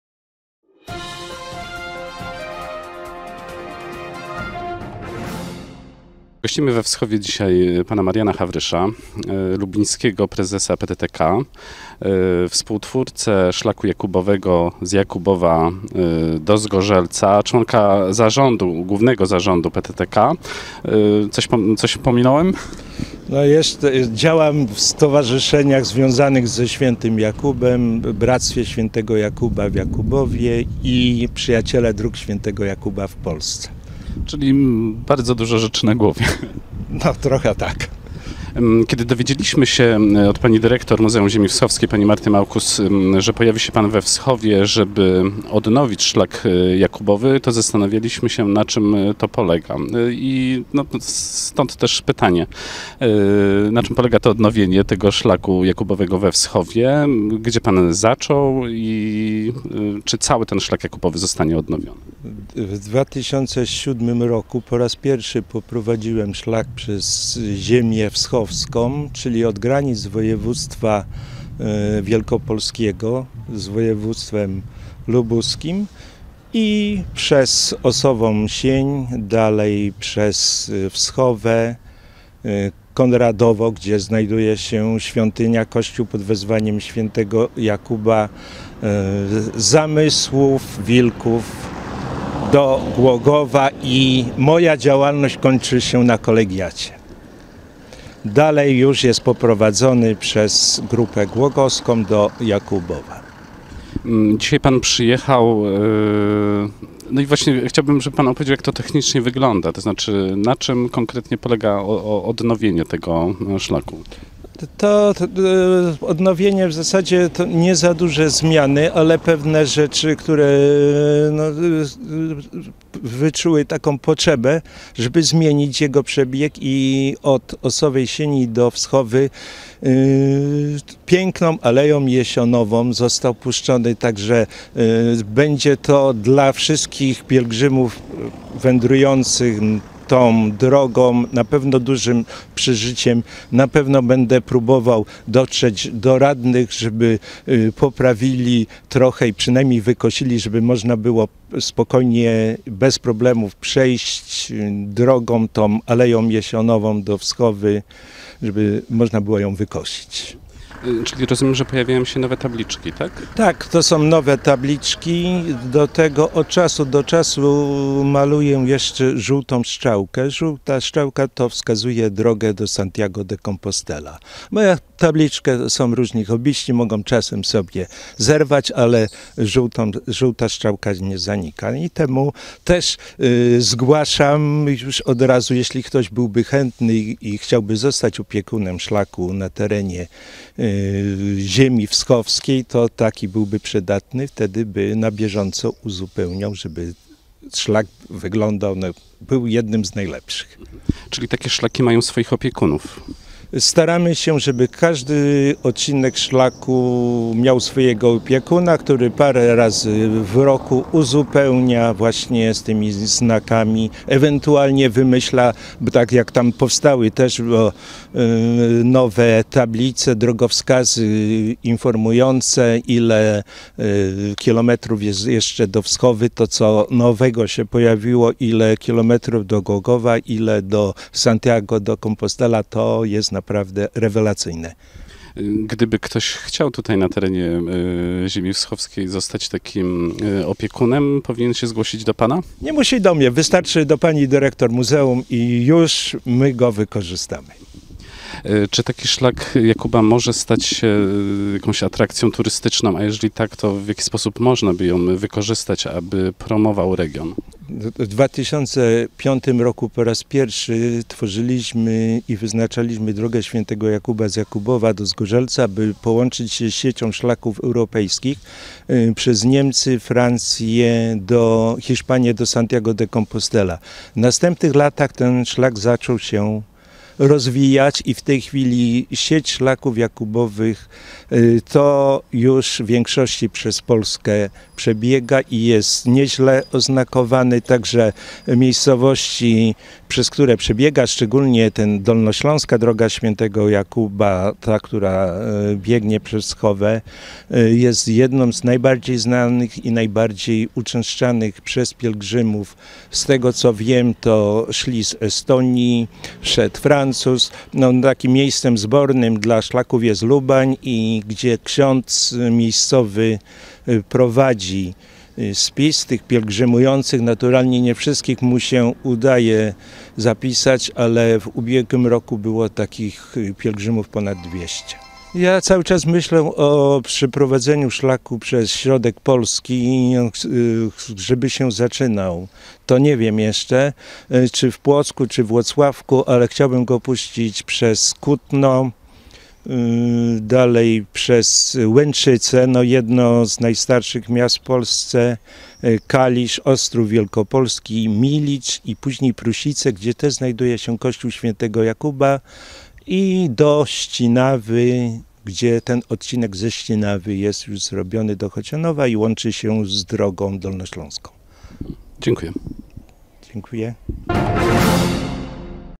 Spotkanie z Marianem Hawryszem we Wschowie
Marian Hawrysz, to osoba wyjątkowa. Wieloletni górnik kopalni Rudna, prezes lubińskiego PTTK, członek Zarządu Głównego PTTK, twórca szlaku św. Jakuba, prowadzącego z Jakubowa do Zgorzelca, czynnie udziela się w stowarzyszeniach, związanych z postacią św. Jakuba
I to właśnie szlak św. Jakuba sprawił, że 24 lipca Marian Hawrysz pojawił się we Wschowie z misją odnowienia drogi apostoła, zakładając tabliczki, symbolizujące szlak oraz rysując żółte strzałki.
W 2007 roku po raz pierwszy poprowadziłem szlak przez Ziemię Wschowską - mówi Marian Hawrysz - czyli od granic województwa lubuskiego i wielkopolskiego przez Osowę Sień, Wschowę, dalej przez Konradowo, gdzie znajduje się kościół pw. św. Jakuba, potem w kierunku Zamysłowa, Wilkowa aż do Głogowa.
Szlak wytyczony przez pana Mariana kończył się na głogowskiej Kolegiacie.
Pobyt pana Mariana w dużej mierze polegał na odnowie szlaku, ale również na wytyczeniu z Osowej Sieni do Wschowy nowej drogi.
Postanowiłem zmienić przebieg szlaku z Osowej Sieni do Wschowy, wybierając piękną aleję jesionową - opowiada Hawrysz - myślę, że pielgrzymi, wędrujący tą drogą będę usatysfakcjonowani pięknem tego miejsca.
Pan Marian przekonuje nas, że warto zostać opiekunem drogi św. Jakuba na terenie Ziemi Wschowskiej.
Dobrze by było - mówi twórca szlaku - aby ktoś chętny podjął się opieki. Można wtedy na bieżąco uzupełniać ją znakami, a dzięki temu sama droga będzie stawała się atrakcyjniejsza.
Żeby zostać opiekunem lokalnego szlaku, wystarczy zgłosić się do Marty Małkus dyrektor Muzeum Ziemi Wschowskiej, a jak zapewnia Marian Hawrysz, wspólnie wytłumaczą takiej osobie, w jaki sposób opiekować się drogą.
Staramy się - mówi Hawrysz - aby każdy odcinek miał swojego opiekuna. Taka osoba kilka razy do roku uzupełnia tabliczkami drogę. Można na różne sposoby uatrakcyjniać odcinek, stawiając tabliczki z informacją ile kilometrów zostało do Wschowy, do Głogowa czy do samej katedry Santiago de Compostela, która jest celem pielgrzymek.